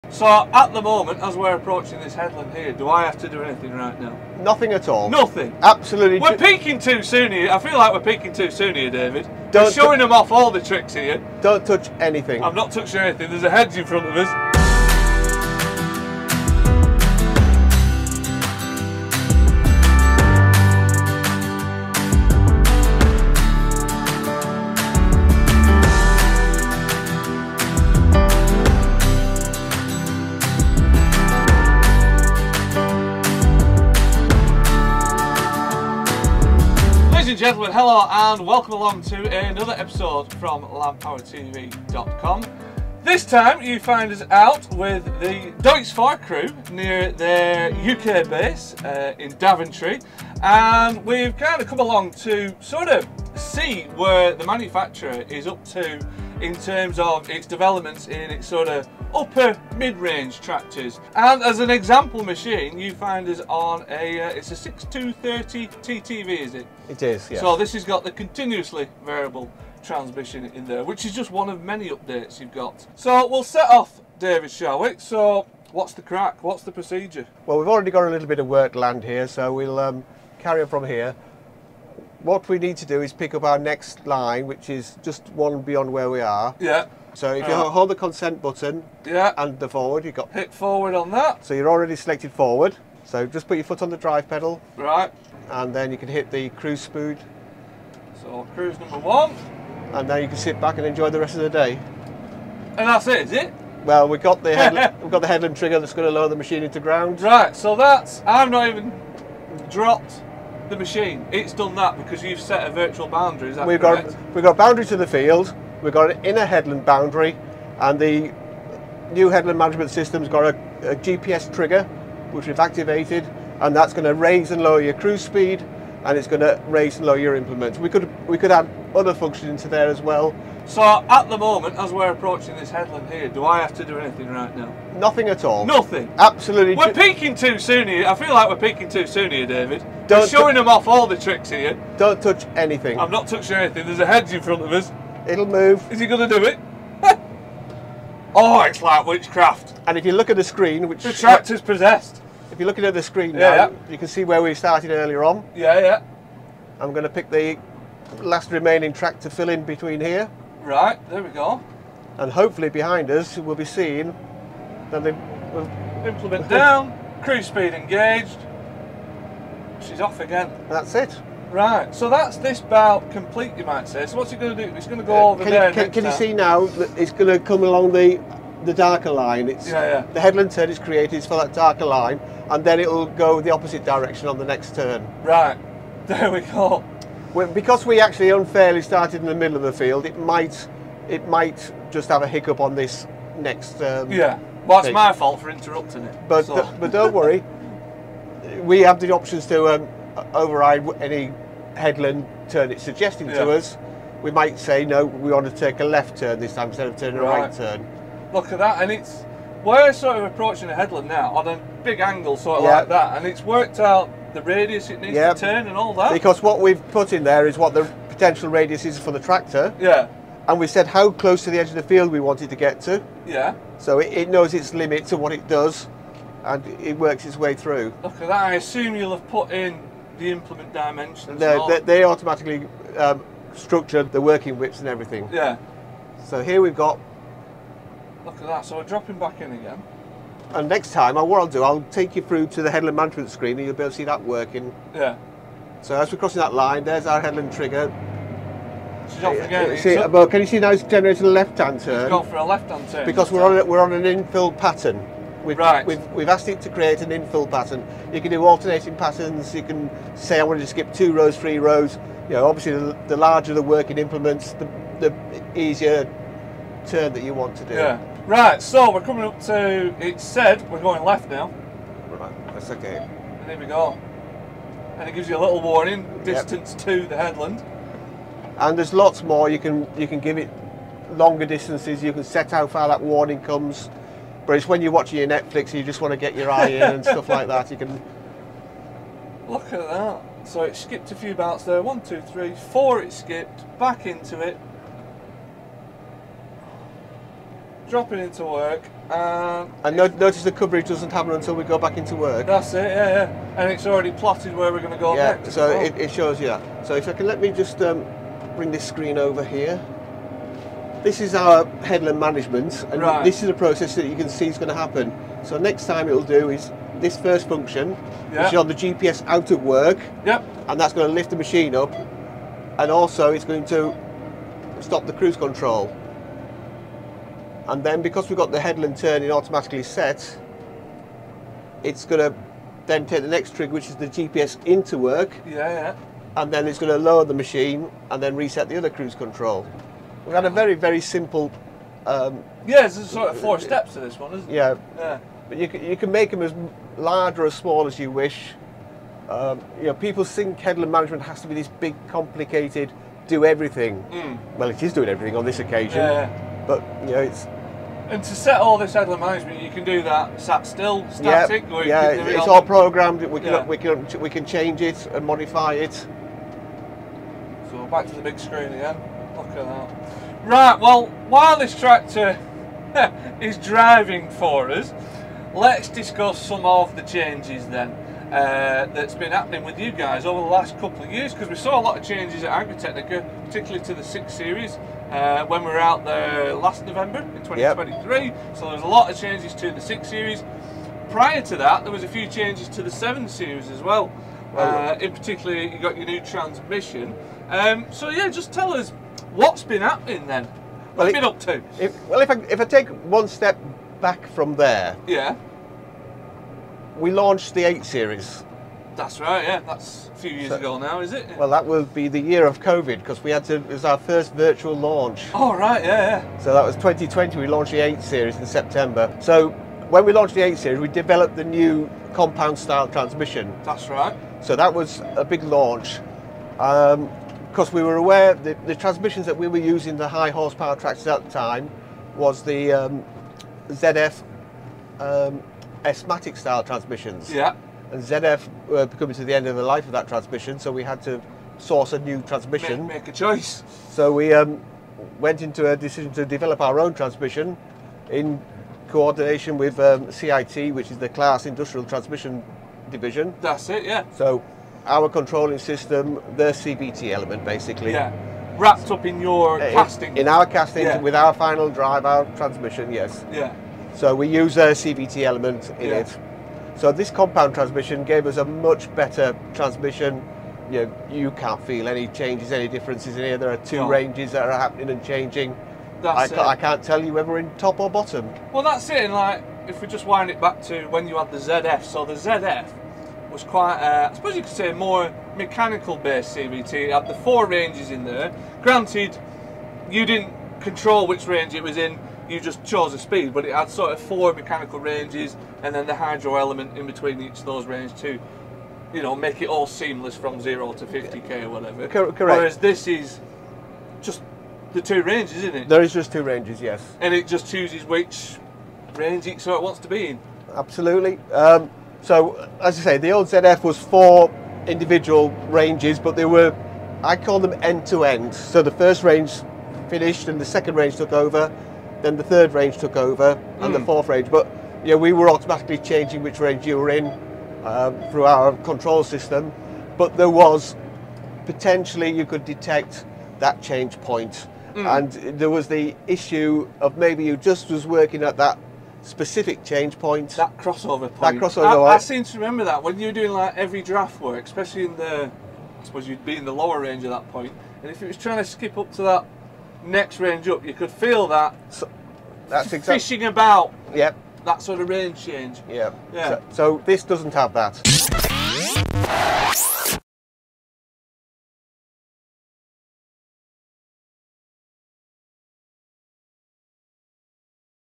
FIRST IMPRESSION: Deutz-Fahr 6230TTV tractor and technology developments
From transmissions to automated headland management systems, we check out a whole host of new tractor developments from Deutz-Fahr.